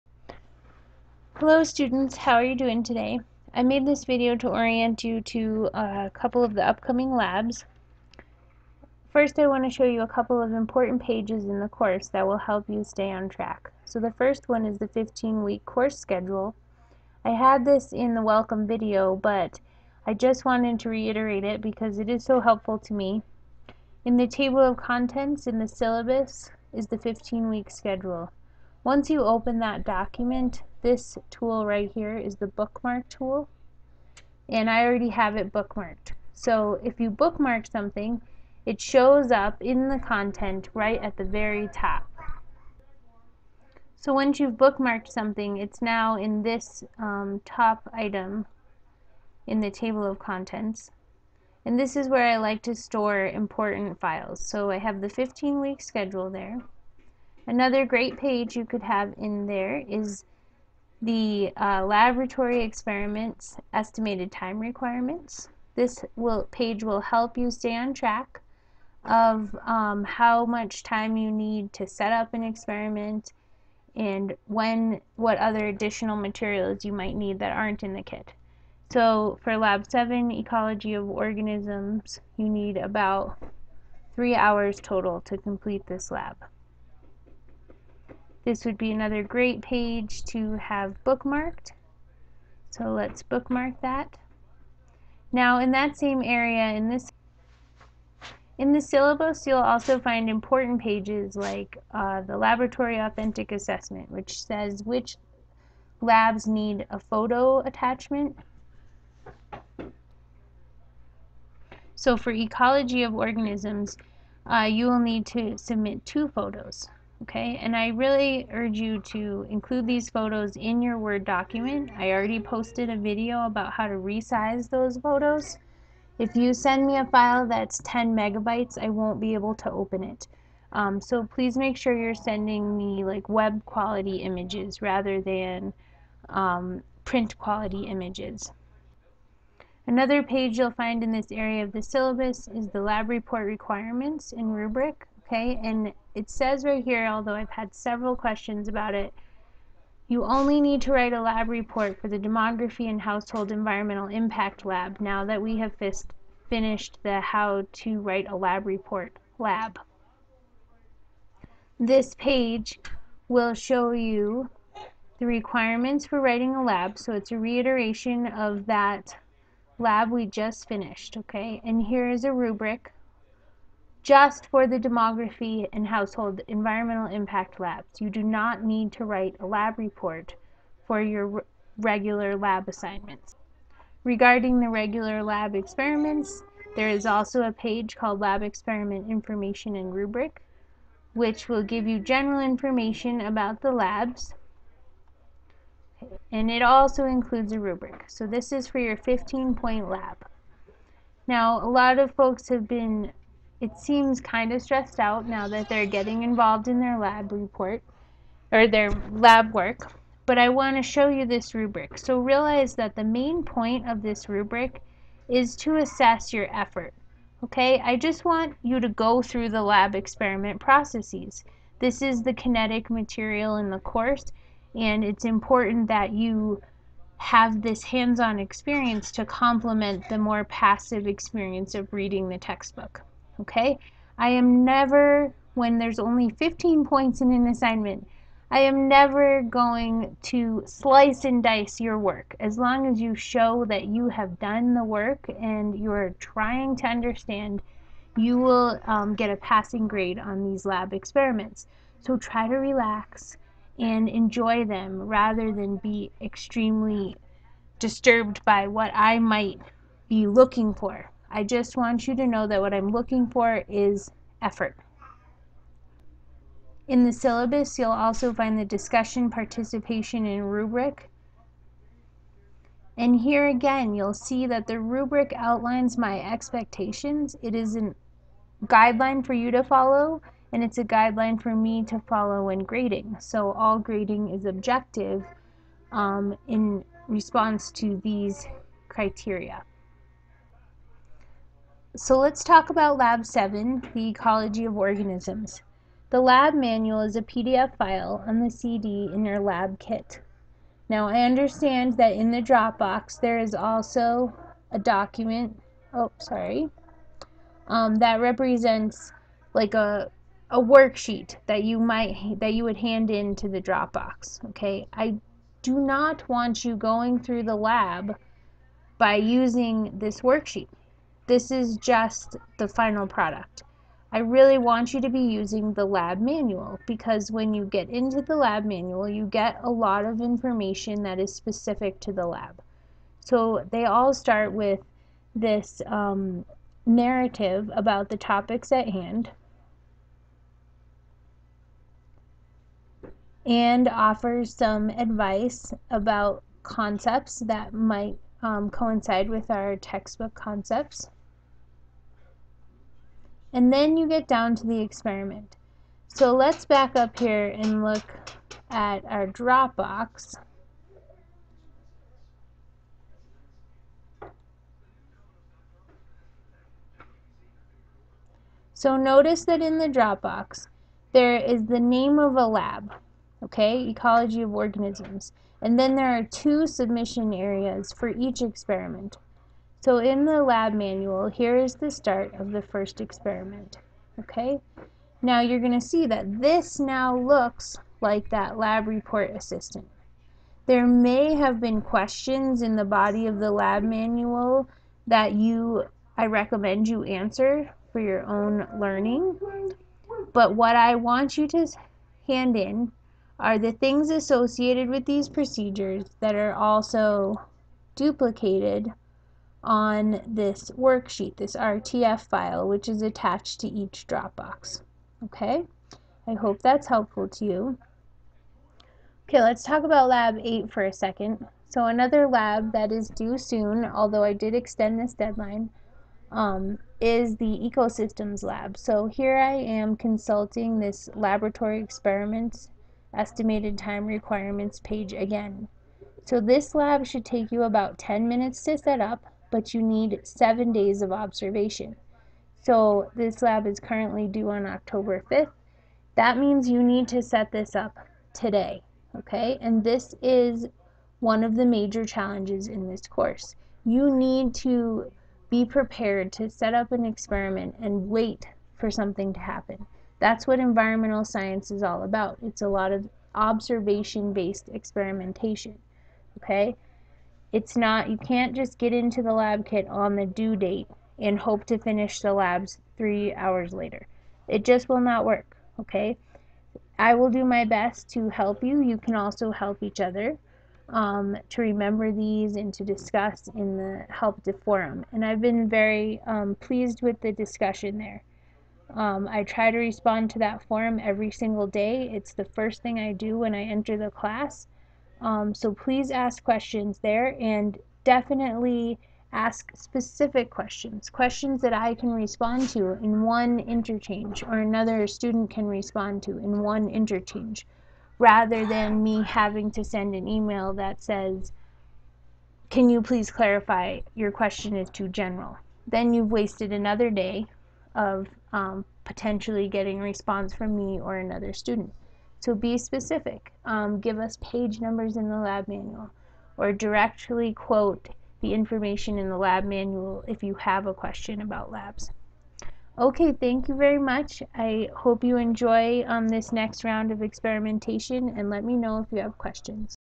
General Lab Information and Advice
In this video, I will review the important syllabus pages that relate to labs. I will also discuss lab procedures and submission, using the Ecology of Organisms and Ecosystems labs as examples. At 7:25 I accidentally say you need 10 minutes to set up the Ecosystems lab but as you will see, the page says 60 minutes. You need 60 minutes.